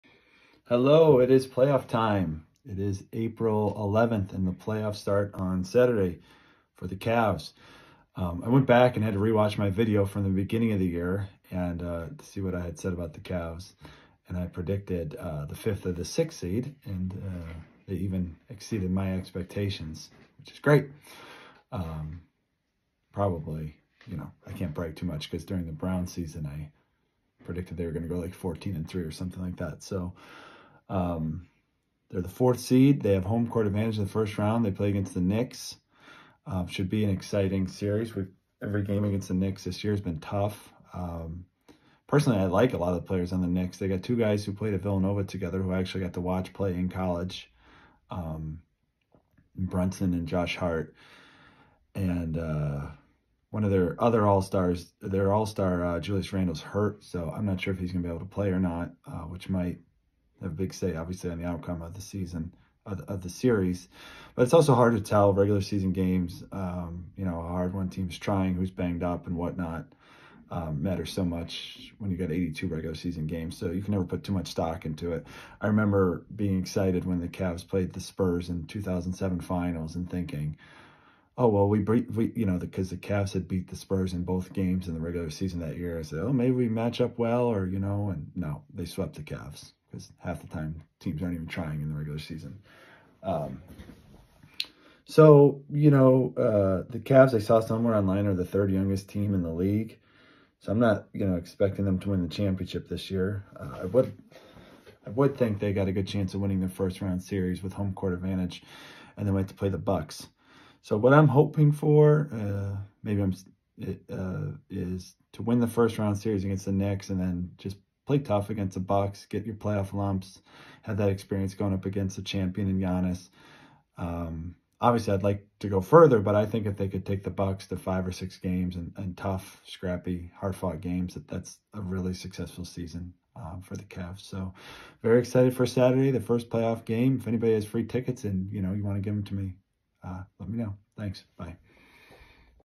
Playoff time is here
We’re just days away from the first Cavs playoff game in 5 years. Can’t wait!